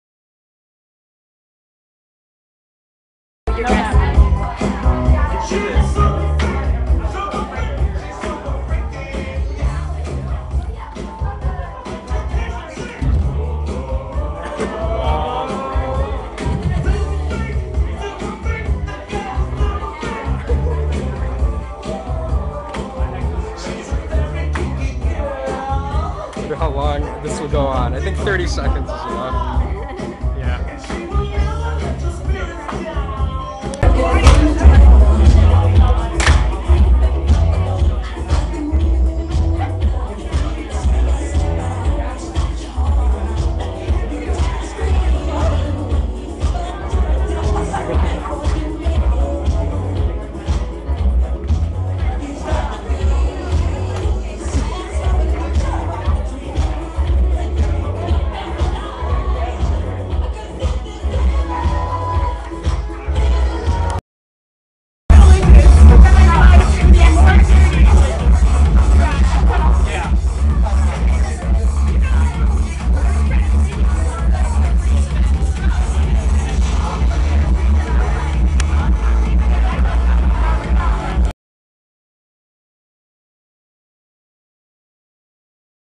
ECUSA: Province V gathering "Episco-Disco"
Province V Episcopal Campus Ministry in the midwest.
February 30th 2009
Last night!
"Episco-Disco"